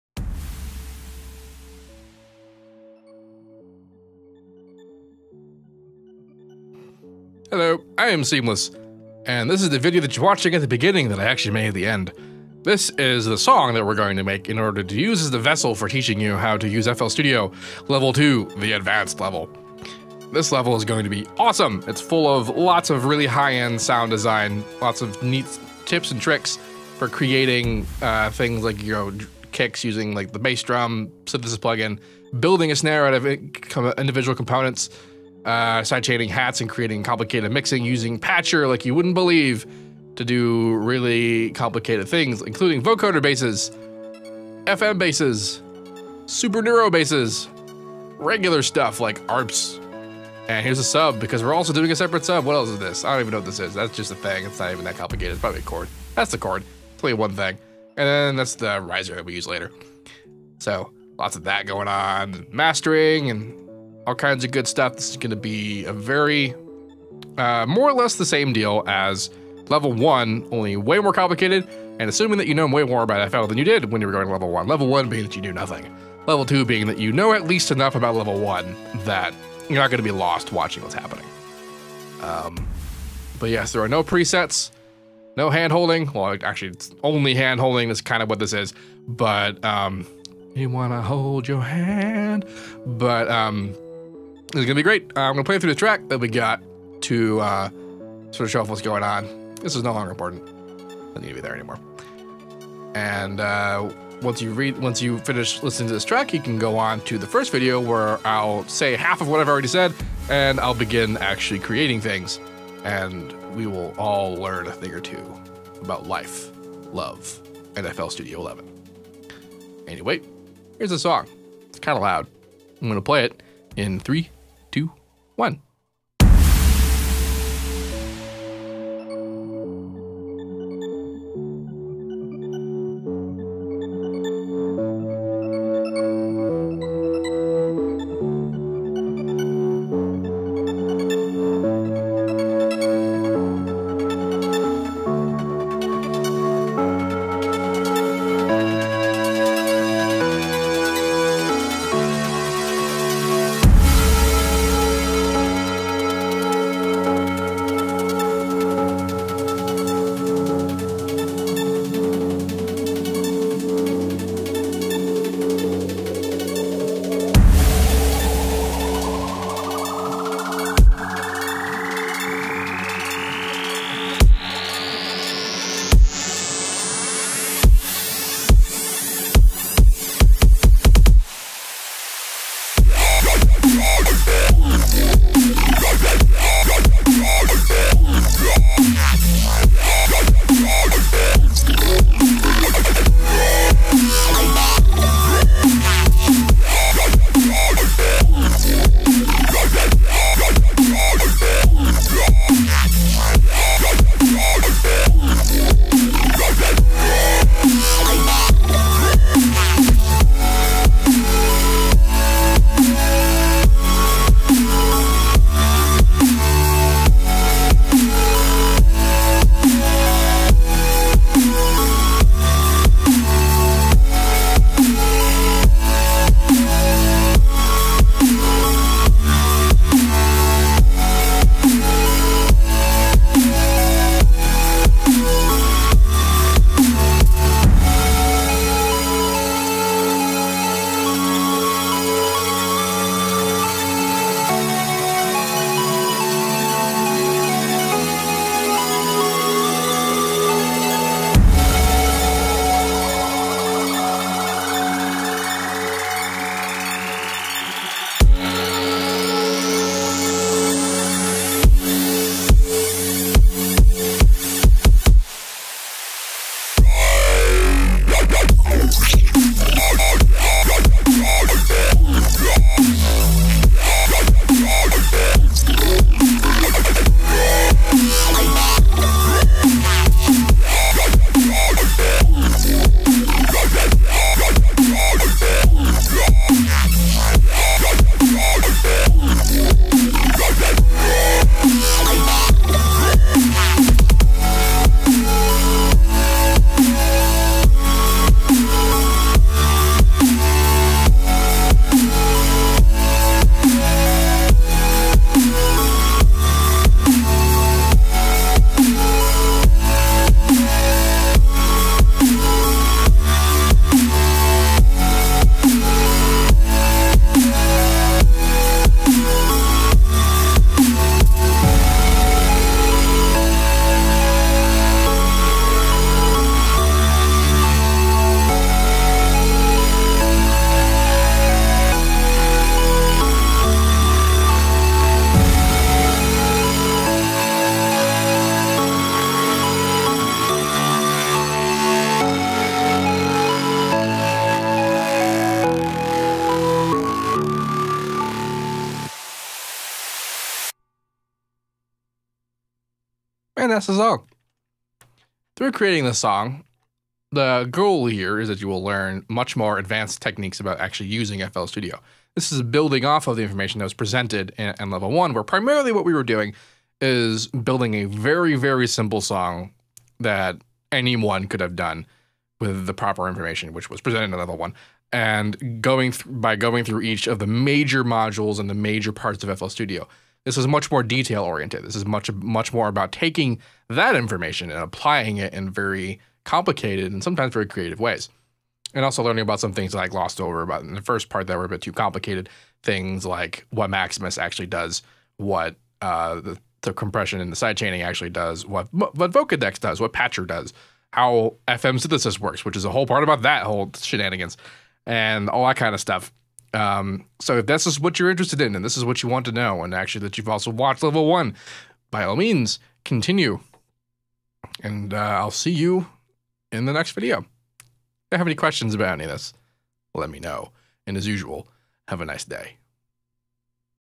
FL Studio 11 Beginners Level 2 - Tutorial 1 - Introduction & Playthrough
Sonic Academy is pleased to present to you our Level 2 ‘Make Music With FL Studio’ with the oracle that is SeamlessR.

This is the next step up from the previous course and one in which you will get to grips with sound design using Harmor for Subtractive synthesis, Sytrus for some good old FM synthesis, and reams and reams of automation for that pro level sound.

Things can get a little complex along the way, but this course will show the full full potential of FL studio right out of the box. So roll up your sleeves and get stuck in.